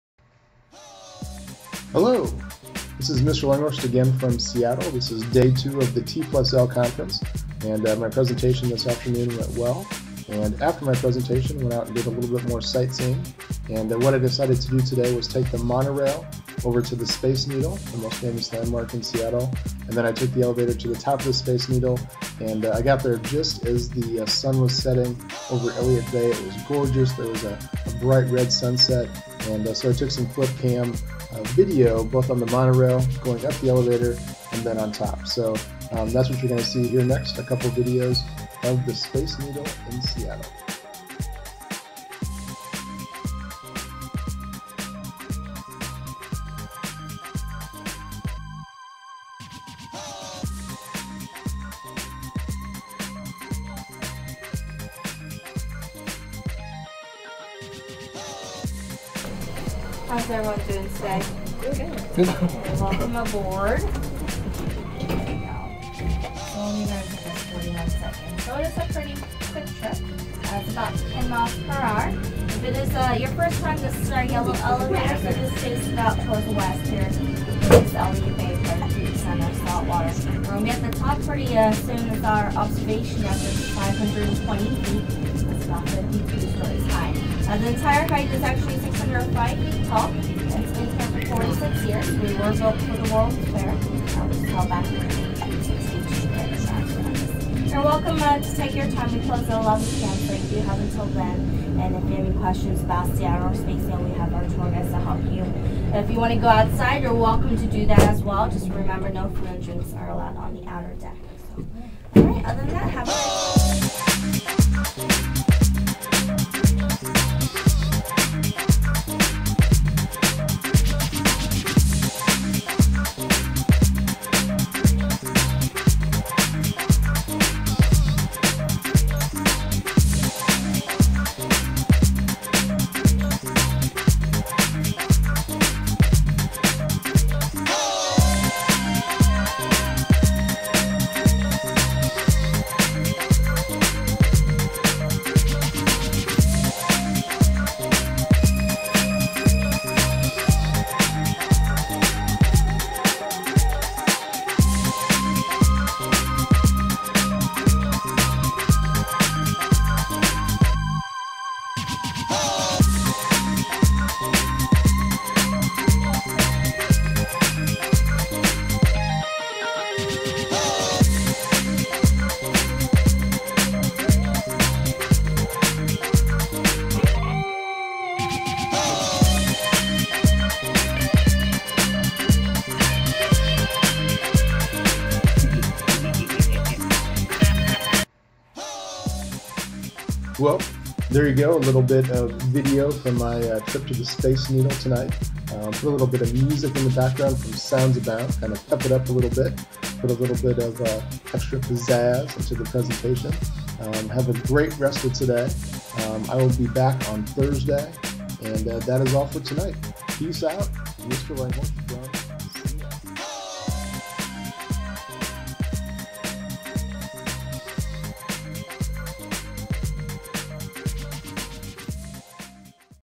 Space Needle in Seattle
A visit to the Space Needle in Seattle in October 2007.